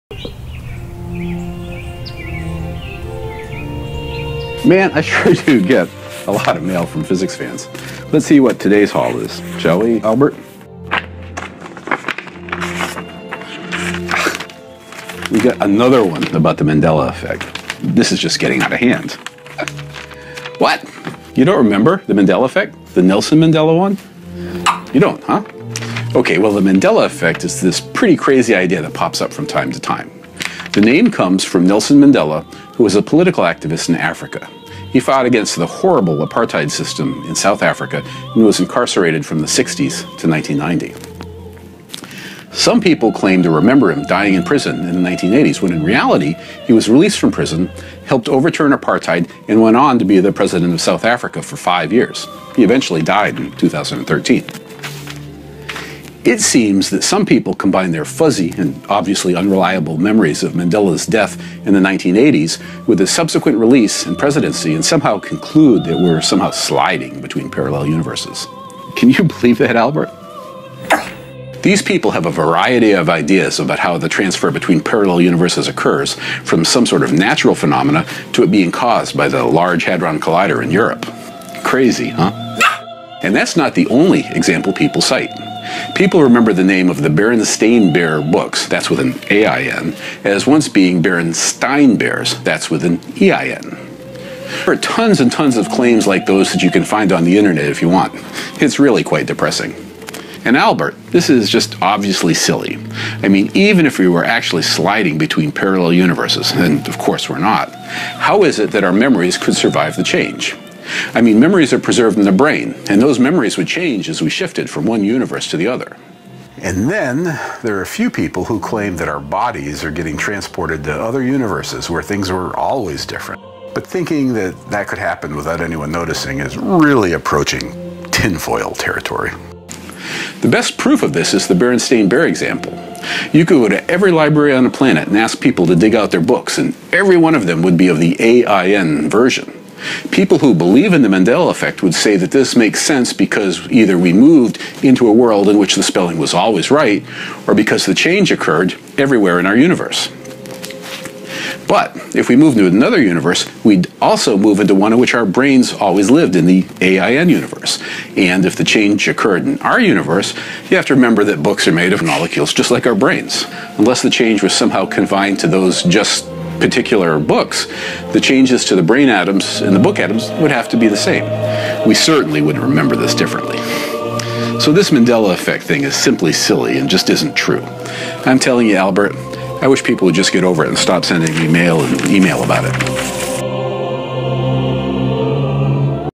ماهو تأثير مانديلا ؟
تأثير مانديلا هي فكرة أن الناس يتنقلون بين أكوان متوازية. نشأ اسمها لأن بعض الناس لديهم ذكريات راسخة عن وفاة نيلسون مانديلا في السجن، بينما في الواقع كان قد أطلق سراحه وأصبح رئيسًا لجنوب إفريقيا. في هذا الفيديو، يشرح الدكتور دون لينكولن من فيرميلاب لحيوانه الأليف ألبرت سبب كونها فكرة مضحكة